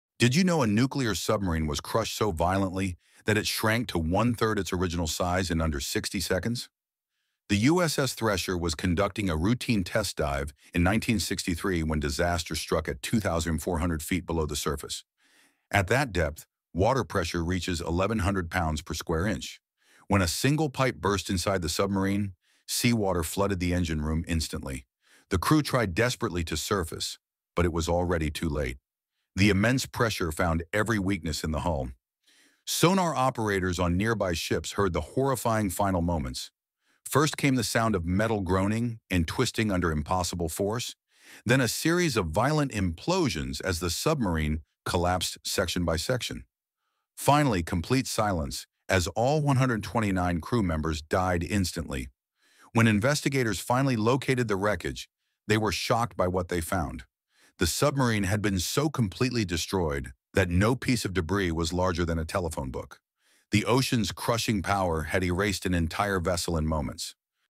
Nuclear Sub Crushed to 1/3 Size in 60 Seconds - USS Thresher Disaster
USS Thresher crushed by ocean pressure in minutes 😱